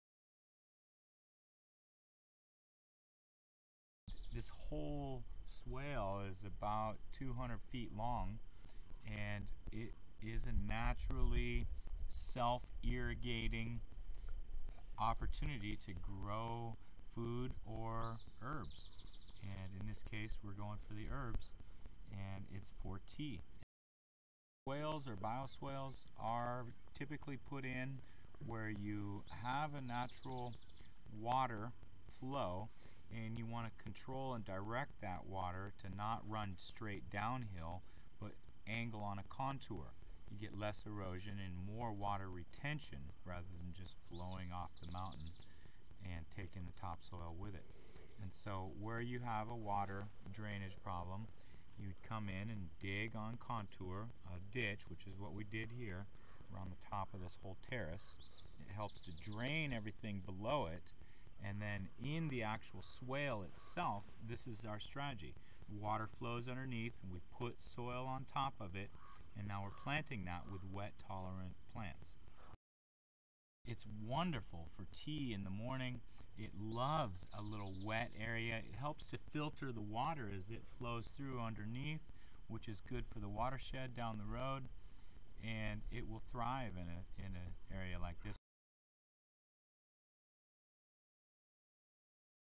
Natural Irrigation: Gardening with Swales
Cougar Mountain Farm's Noah Wemple talks about the how-tos of swales--how they work, and recommendations on what to grow. or search for Brown Dirt Media.